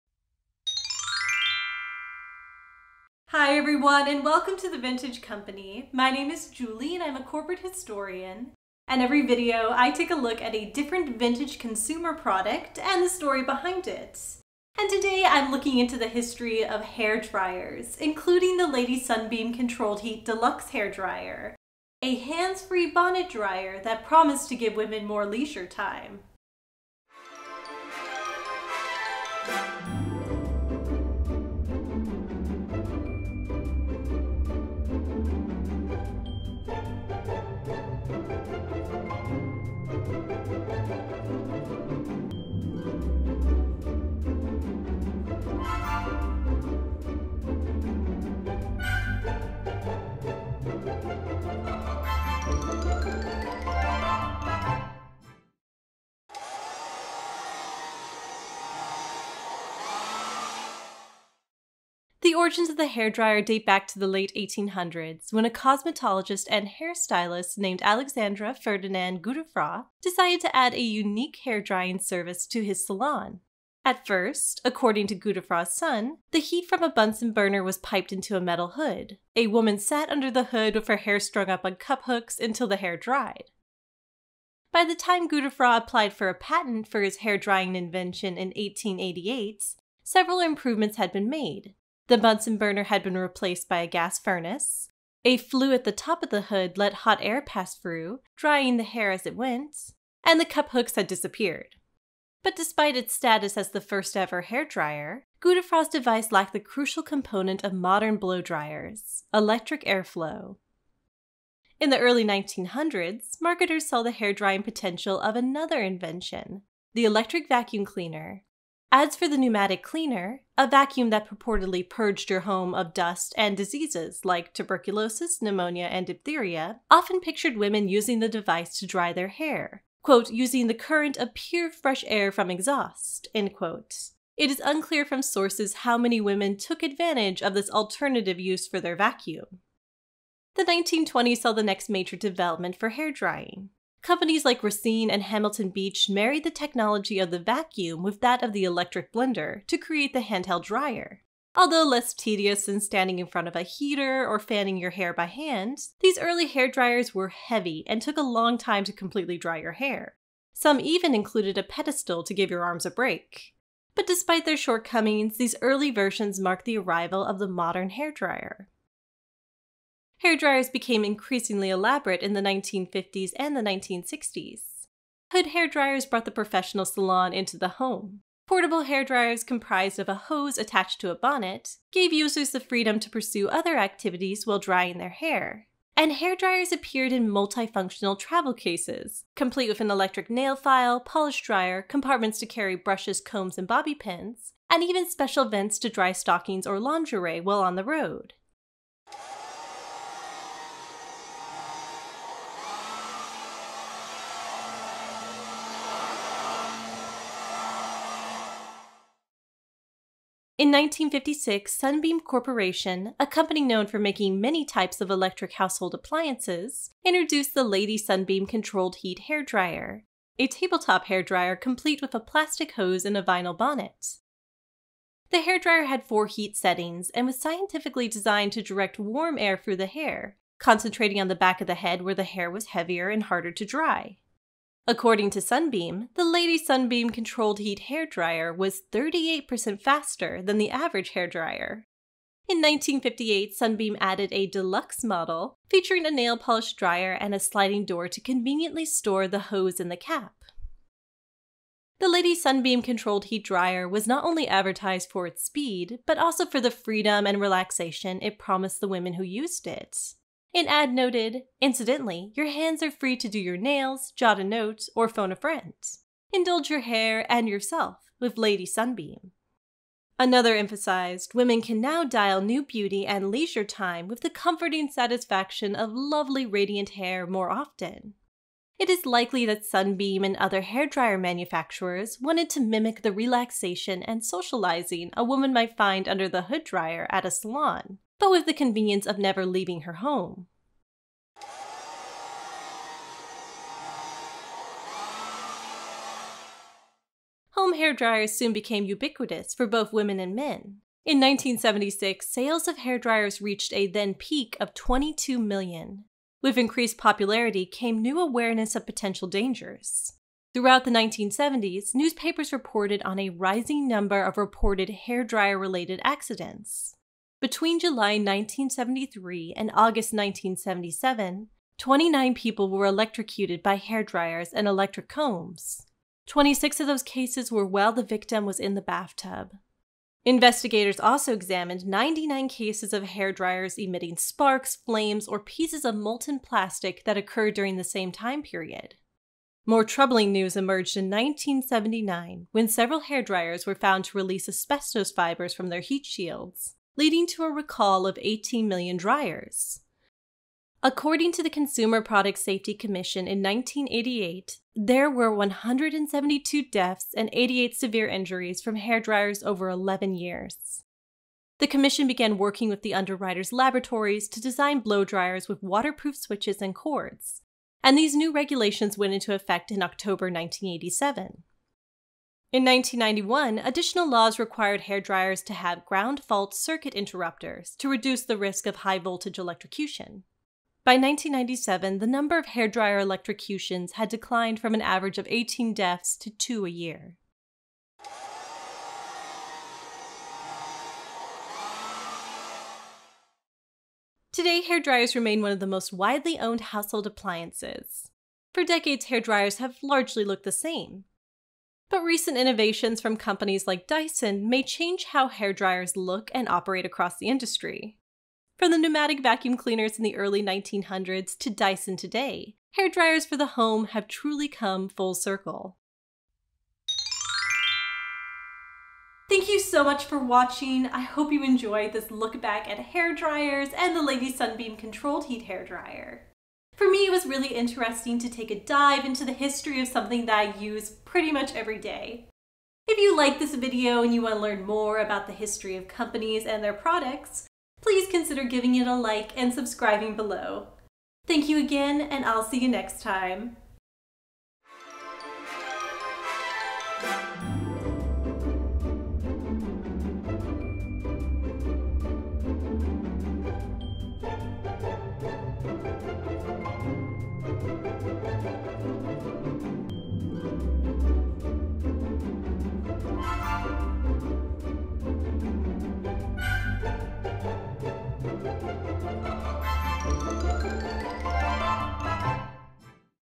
A History of Hair Dryers, ft. the Lady Sunbeam Deluxe Controlled Heat Hair Dryer
In this video, I take a look at the history of hair dryers, from the first hair dryer in the late 1800s to today, with a special look at the multifunctional Lady Sunbeam Deluxe Controlled Heat Hair Dryer, a bonnet dryer and nail polish dryer that promised to give women more leisure time.

Sources:

“The Hair Dryer, Freedom’s Appliance,” Maria Teresa Hart, The Atlantic, Aug 1, 2017

“Who Made That Hair Dryer?” Jessica Gross, The New York Times Magazine, Jul 19, 2013

The Housewares Story: A History of the American Housewares Industry, Earl Lifshey, 1973

“Hair Dryer for Home Use,” News-Journal, Jul 17, 1921

“The Beauty Parlor of Bygone Days,” The Evening Sun, Feb 14, 1946

“Controlled-Heat Hair Dryer Offers New Leisure Time,” The Town Talk, Nov 6, 1956

“Hair Dryers In Home Use Vary Widely,” The New York Times, Jul 3, 1961

“That Hair Dryer Could Be Dangerous,” Jo Thomas, Special to The New York Times, Jan 16, 1978

“The Ubiquitous Hair Dryer Everybody's Got One,” Wendy Schuman, The New York Times, Jul 16, 1978

“Record Level Of Asbestos In Hair Dryer,” The New York Times, Sep 22, 1979

“Consumer’s World: Making a Safer Blow-Dryer: Progress, but at a Snail’s Pace,” Michael Decourcy Hinds, The New York Times, Aug 27, 1988

“Hazardous Hair Dryers,” Don Oldenburg, The Washington Post, May 14, 1997

“Dyson Wants to Create a Hair Dryer Revolution,” Elizabeth Paton, The New York Times, Apr 27, 2016